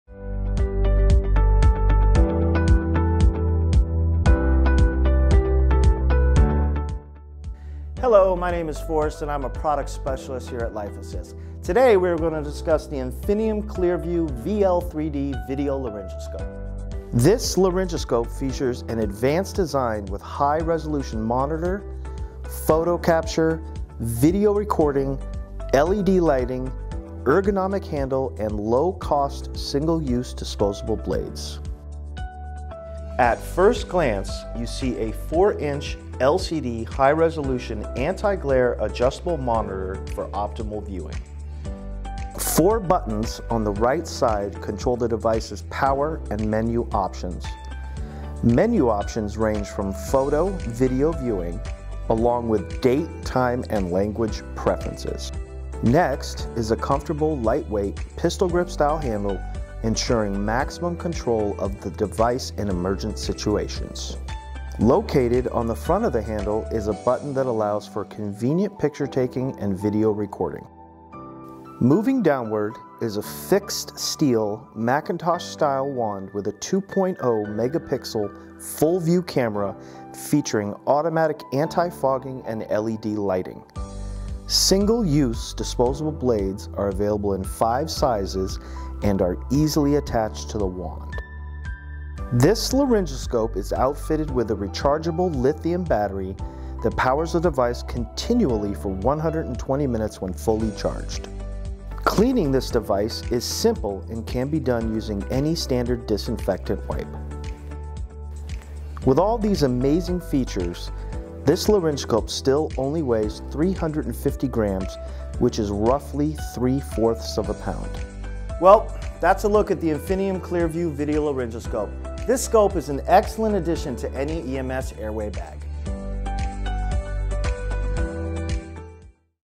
Infinium ClearVue Video Laryngoscope
Watch as our Senior Product Specialist, Forrest takes you through some of the features of the Infinium ClearVue VL3D Laryngoscope.

The Infinium ClearVue® video laryngoscope adopts state-of-the-art image technology and camera with high-resolution monitor. The ClearVue® features a rechargeable li-ion battery for extended use. The overall design is compact and ergonomic. A high-resolution view of the glottis will enable clinicians first attempt success while minimizing any chance of a complication during the intubation process.

Opening (00:00)
Quick Overview (00:16)
Monitor (00:33)
Button Functions (00:43)
Exterior Features (00:56)
In-Use & Weight (1:47)
Review (1:59)